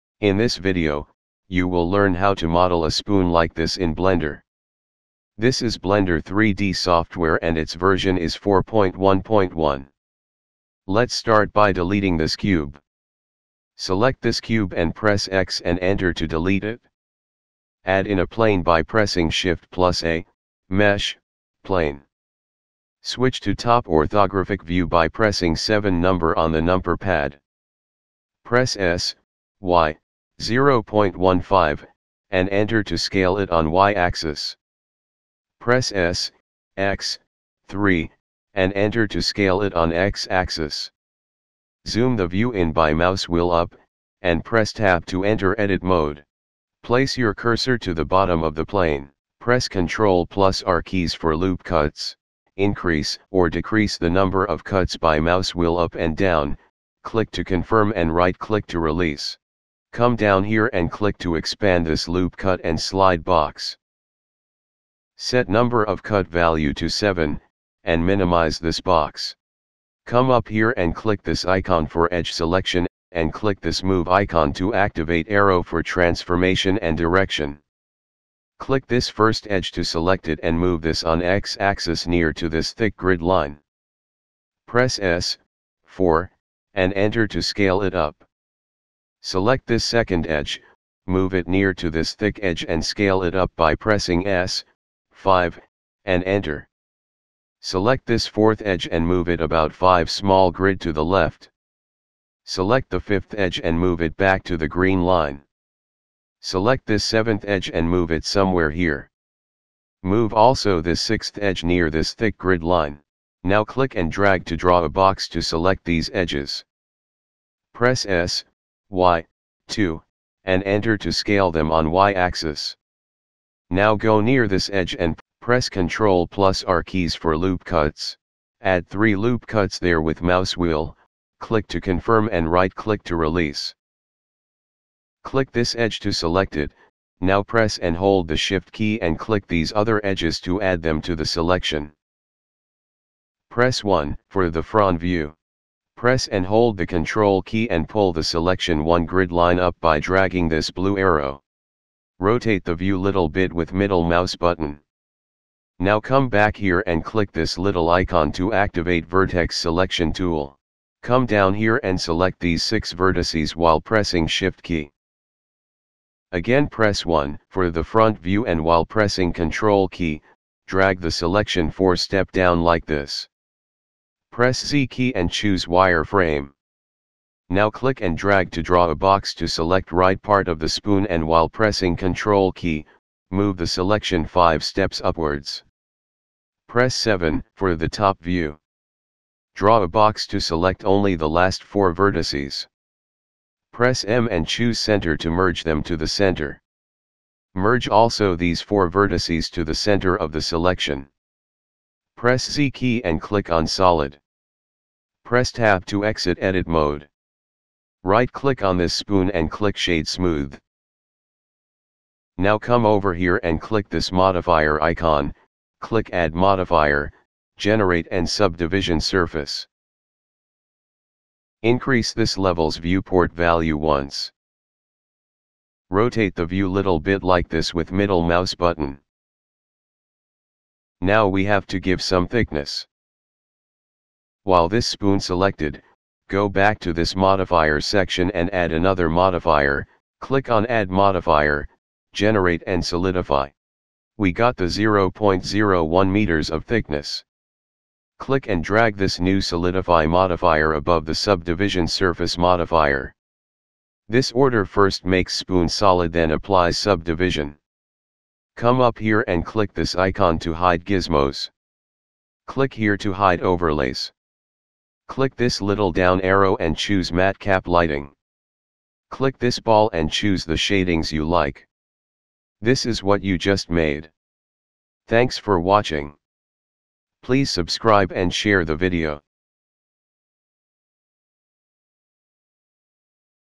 Blender Tutorial - How to model a SPOON in Blender 4.1
In this video, You will learn how to model a spoon in blender.
Blender version 4.1.1 is being used while making this video.

We will cover basics of
1. Object Mode and Edit Mode
2. Loop cuts
3. Transformation of geometry
4. Subdivision surface modifier
5. Solidify Modifier
6. Some viewport setting.